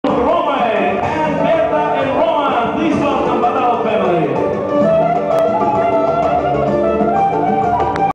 006.MPG, Misha Normatov,
MAZAL TOV - WEDDING OF EMANUEL ABAYEV AND SHARONA SHIMUNOV IN 'Rn DAMIKEL ILLAGIO"
QUEENS, NY, 07/03/2014